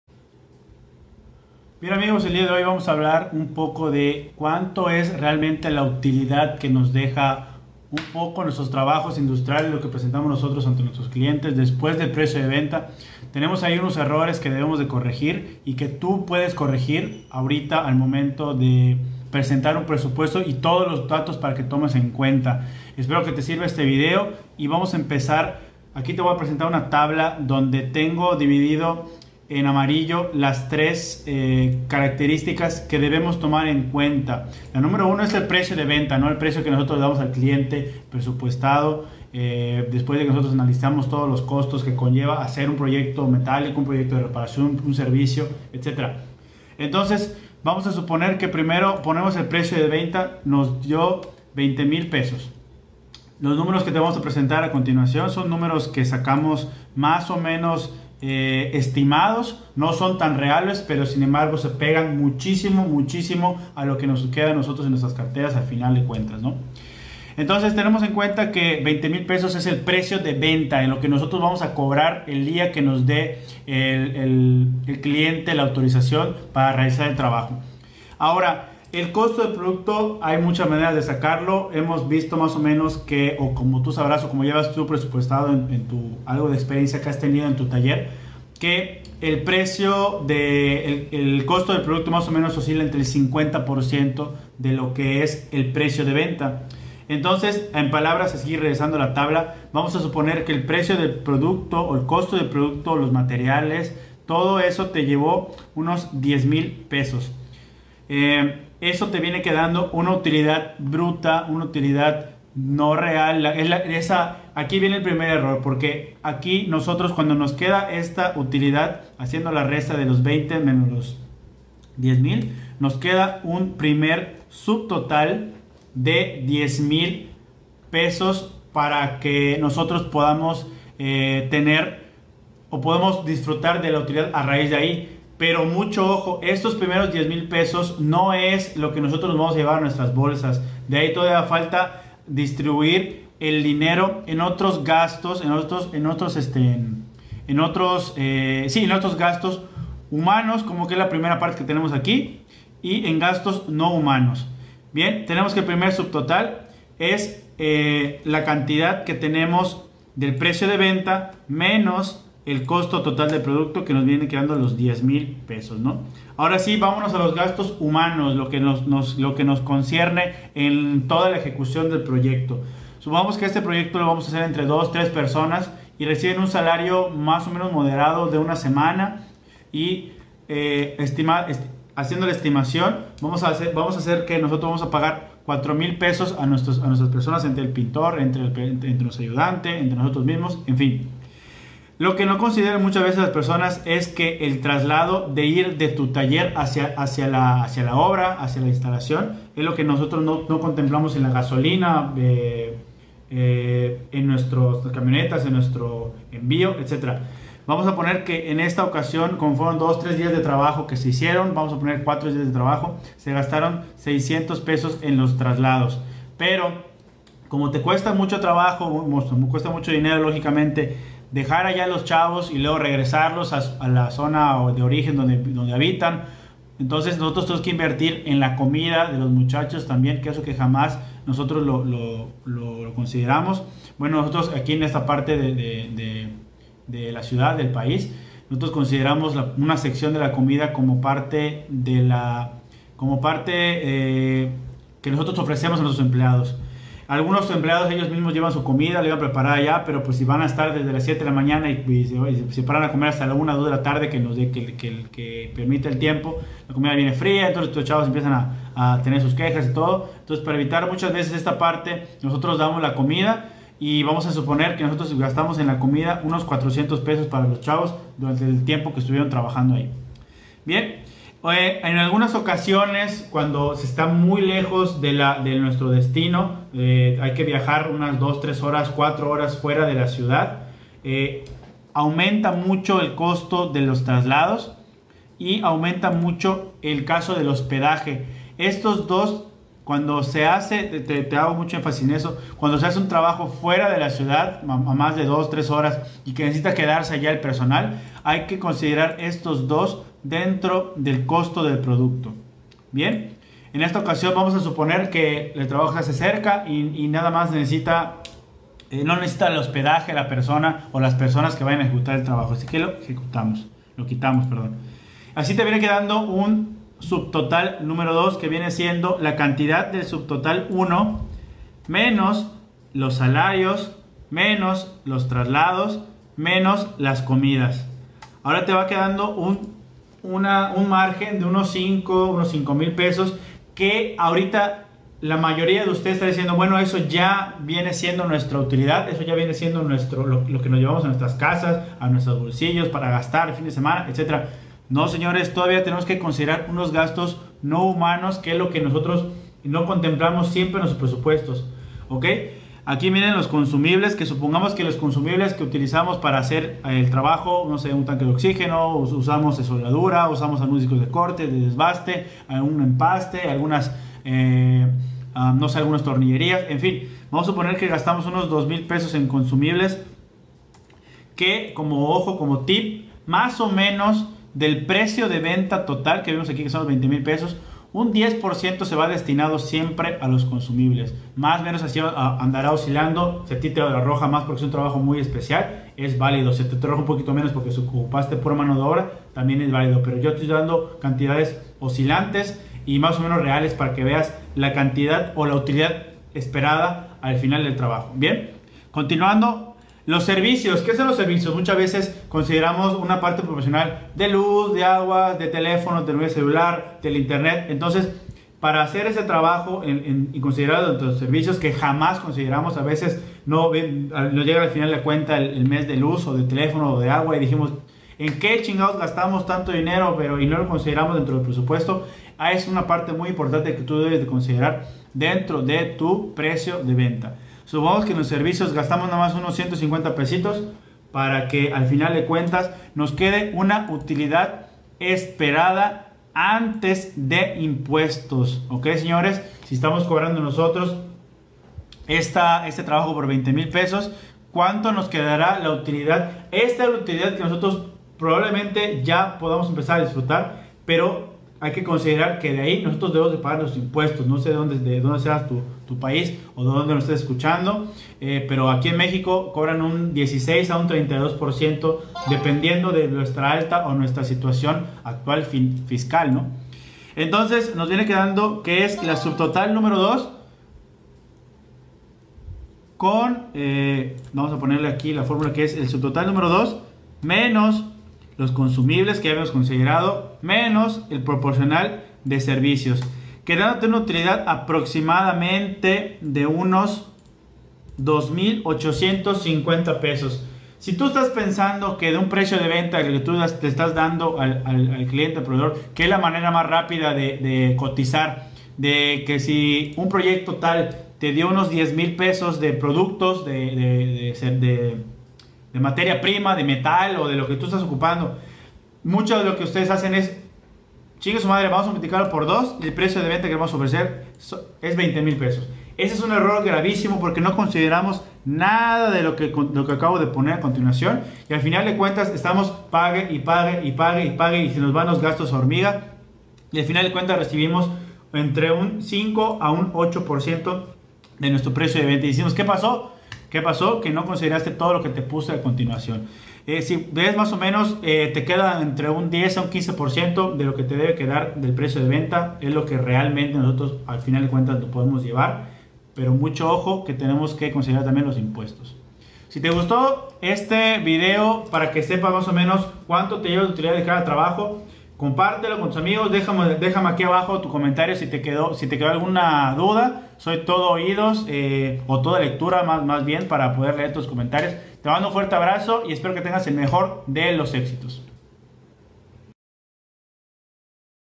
¿Cuál es la utilidad real de un trabajo?
El peor error de un negocio es no saber cuanta lana te deja realmente un trabajo. Descubre el por qué no estás ganando lo suficiente.